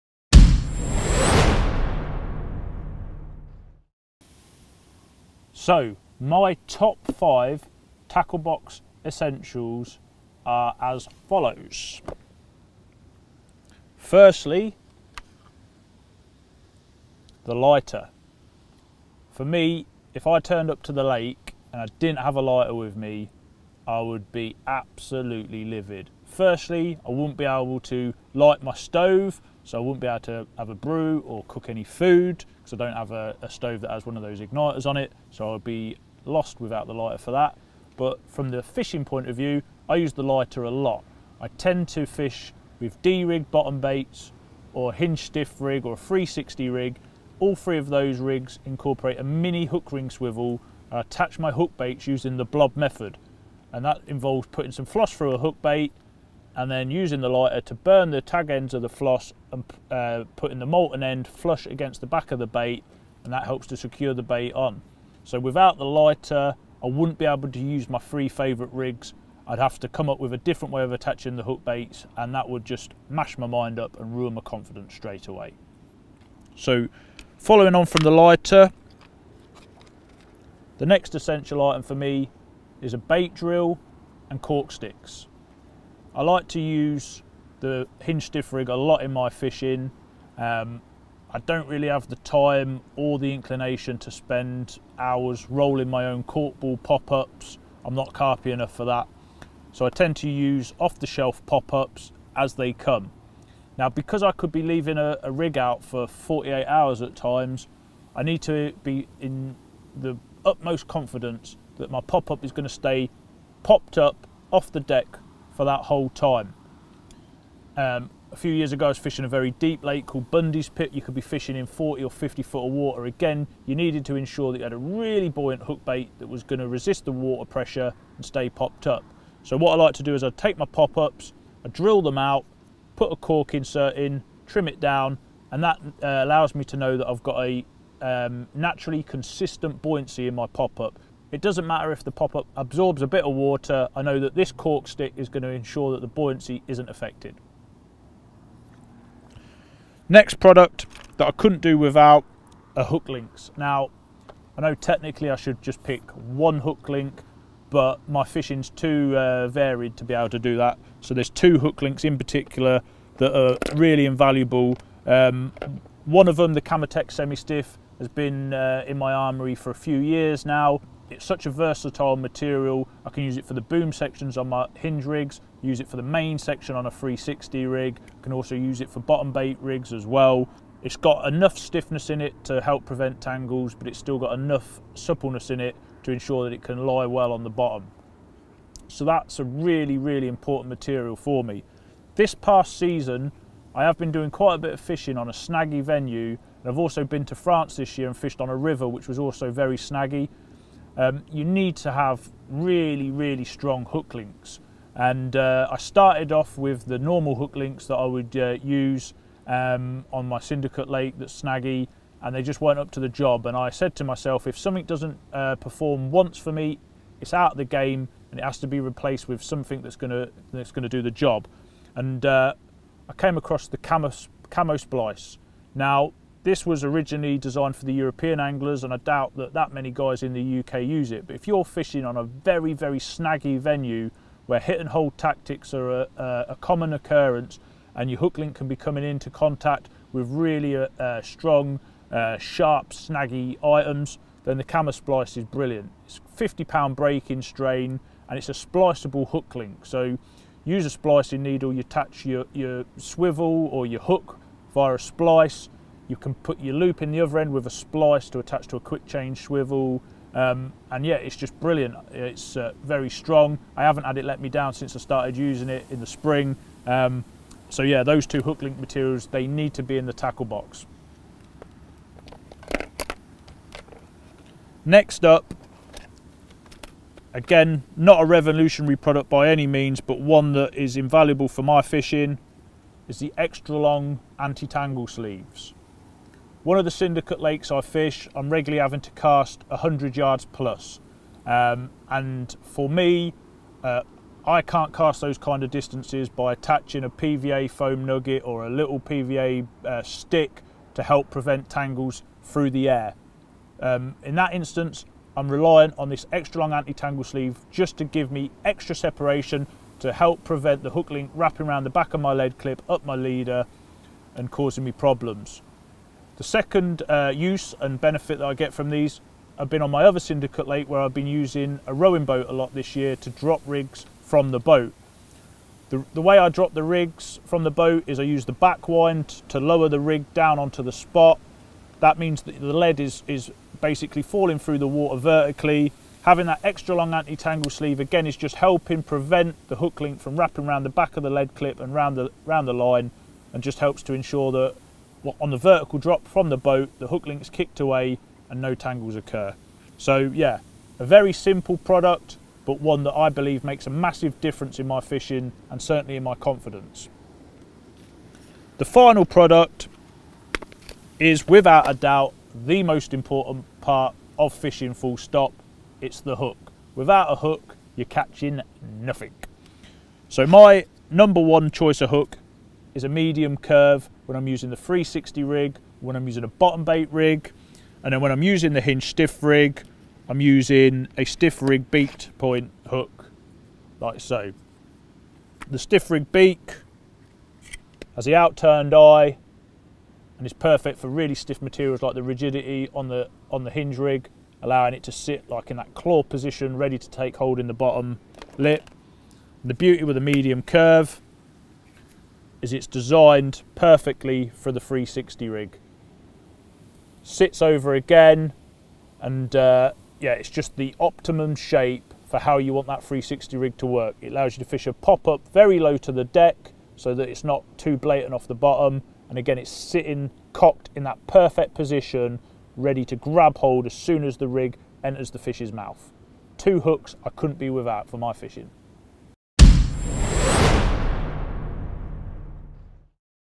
***CARP FISHING TV*** 5 Tackle Box Essentials: Lewis Porter
Lewis Porter tells you about the five tackle box items he wouldn't be without and explains why they are such important parts of his armoury.